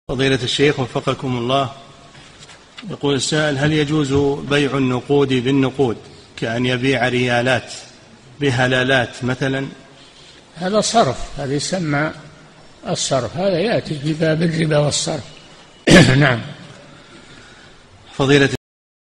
[1540 -3022] هل يجوز بيع النقود بالنقود كأن يبيع ريالات بهلالات مثلاً؟ - الشيخ صالح الفوزان
-"[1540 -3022] السؤال: السائل هل يجوز بيع النقود بالنقود كأن يبيع ريالات بهلالات مثلاً؟
الجَوَابُ: هذا صرف هذا يسمى الصرف، هذا يأتي في بابه في باب الربا والصرف، نعم.
للاشتراك في قناة الشيخ صالح الفوزان

للاستفسار والإقتراحات والدعم
رئيس الفريق
لمشروع كبار العلماء